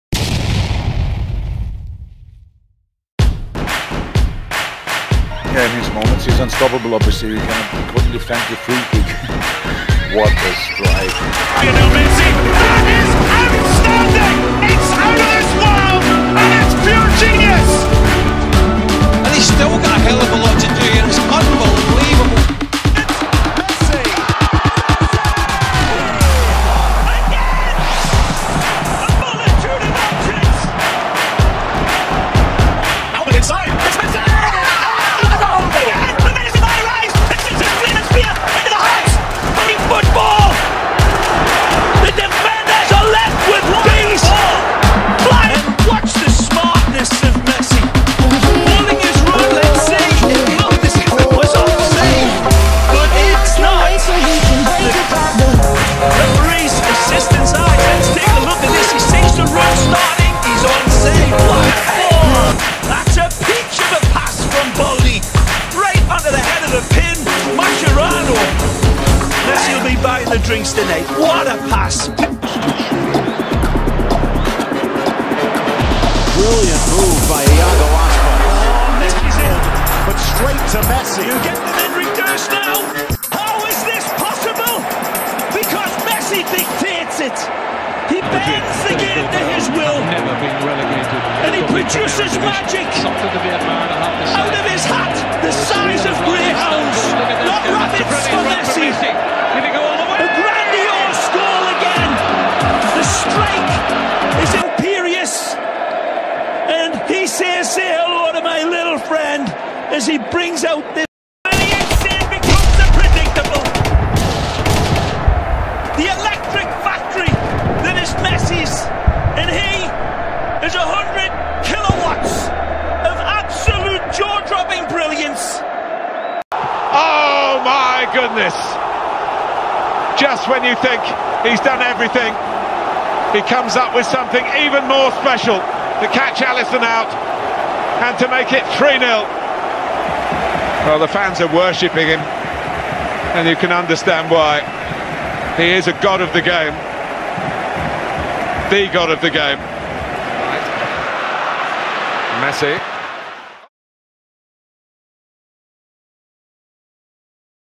Messi Birthday Status | Ray Hudson | Mark Tyler | Messi Skills and Goals with commentary
The warmest wishes to the G.O.A.T. for his 34th Birthday.
This was the day when the alien came out of his space - ship down to the earth. Now he has spent 34 years here and ruled the football world.